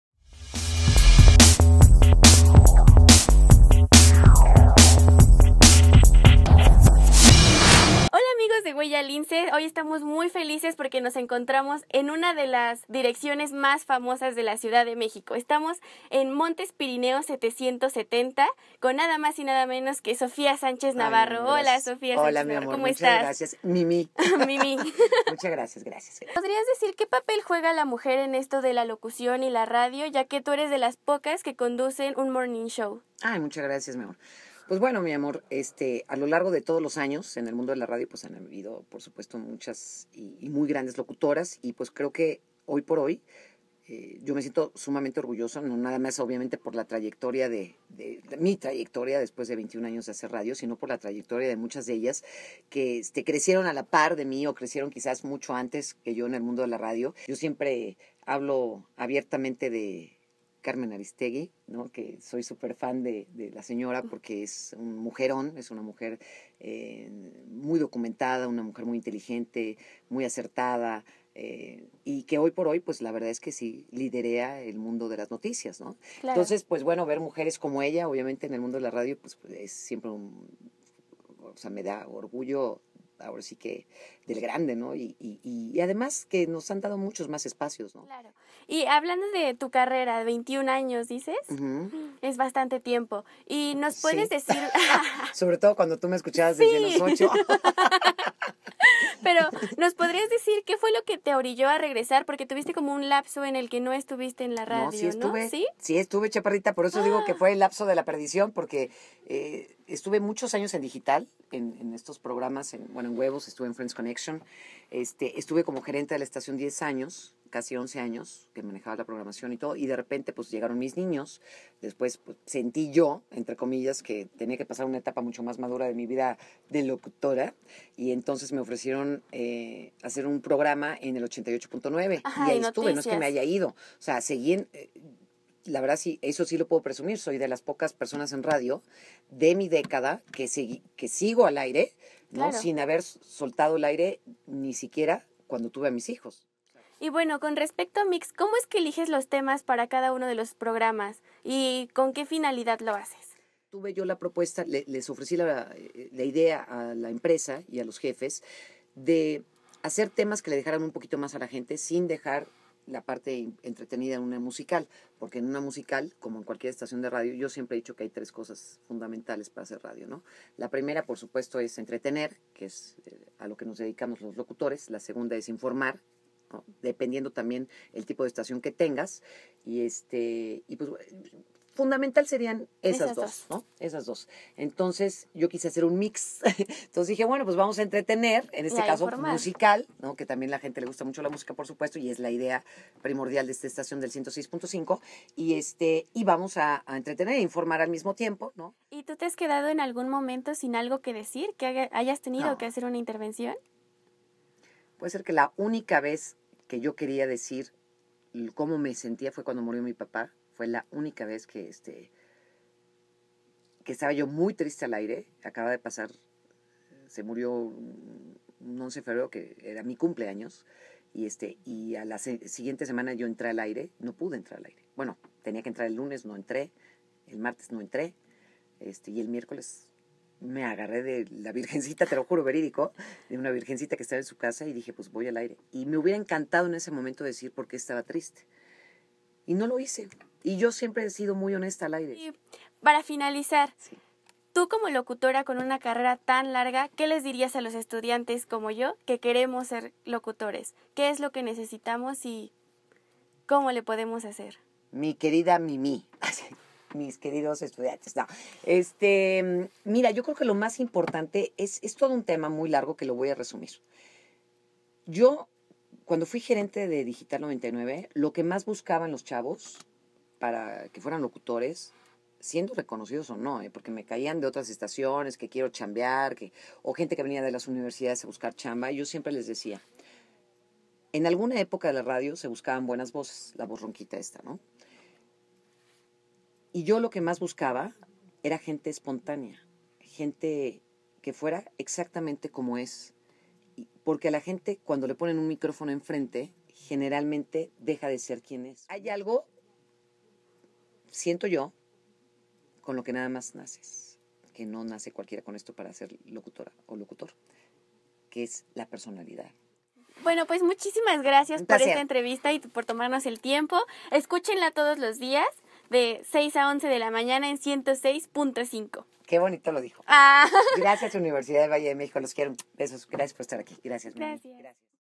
Sintonizando a: Sofía Sánchez Navarro
La voz matutina en Mix 106.5 nos habla del papel de las mujeres en la radio y de todo lo que se necesita para ser un buen locutor.

Entrevistador: Nohemí Silva @whoyouneed
Editor y cámaras: Héctor Núñez @hec55_11

HuellaLince.com